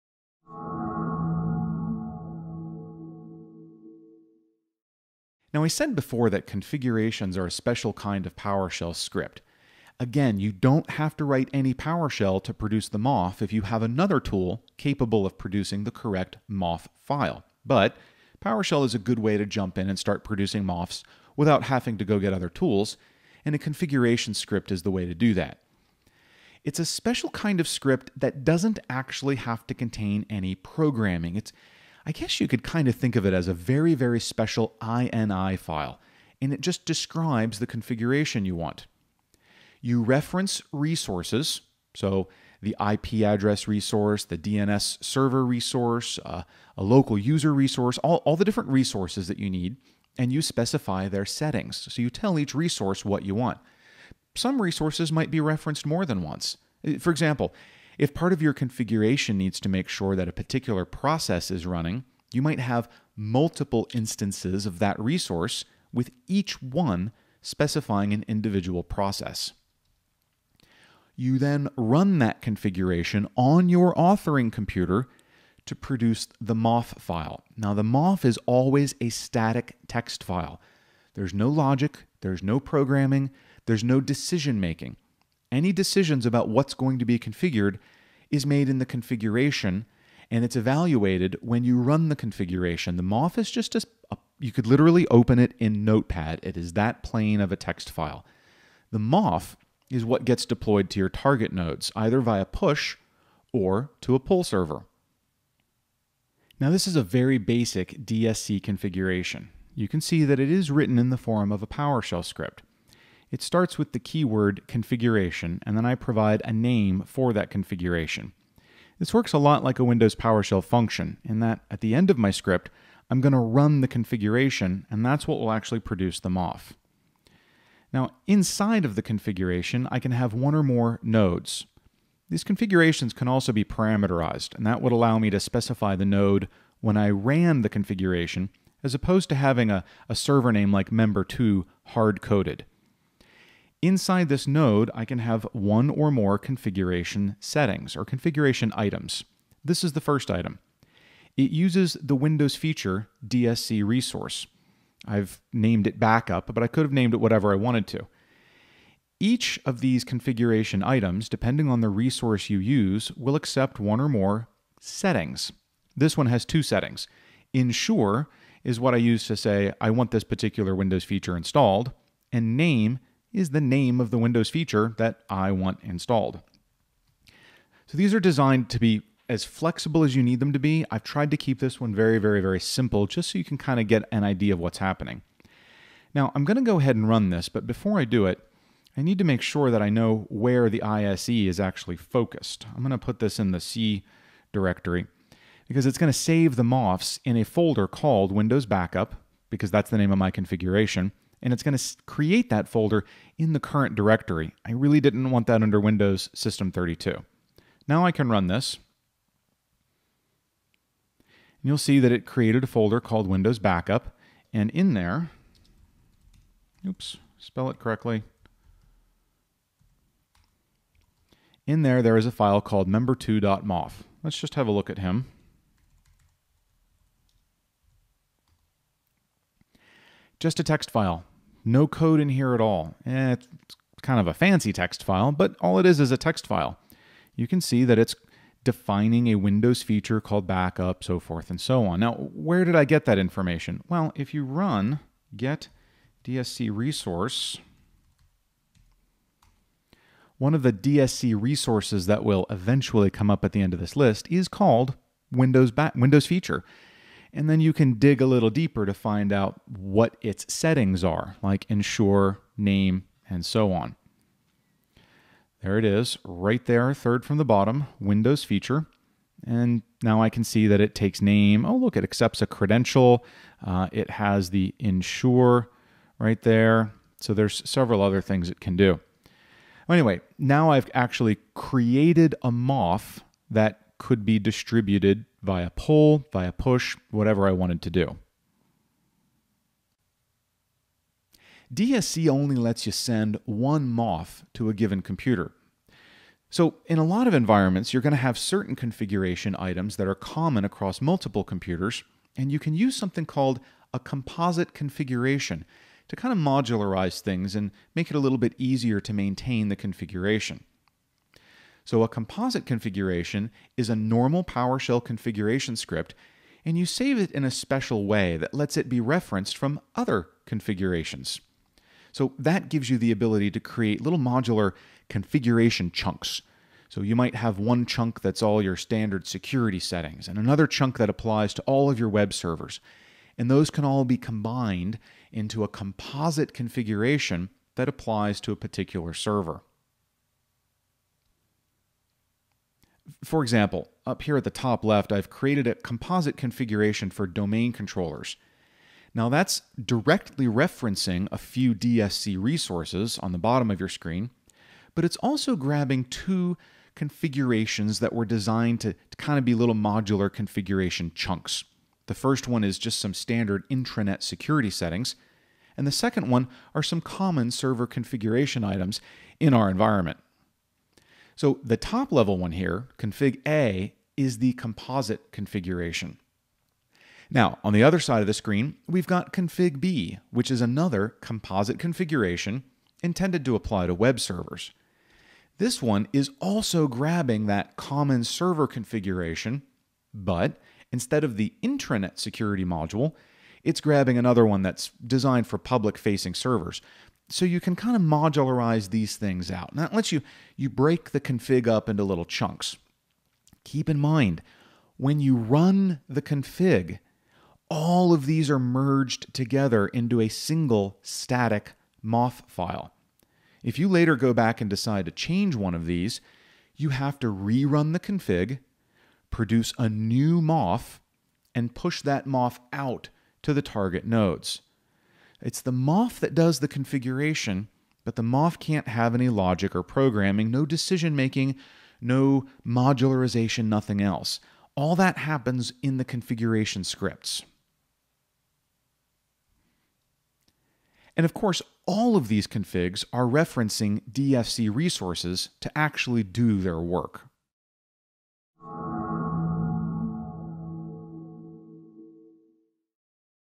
Windows Server 2012 Installing and Configuring (70-410) training
In this free clip from part 4 of his Windows Server 2012 Installing and Configuring (70-410) course, IT expert Don Jones guides you through basic configurations.